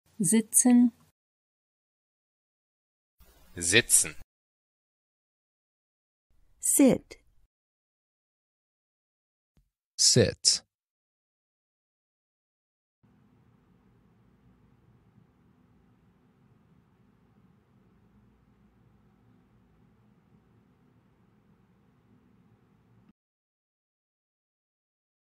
How to say "Sit" in German
Male Voice:
sitzen

Female Voice:
sitzen

Male Voice Translation:
Sit

Female Voice Translation:
Sit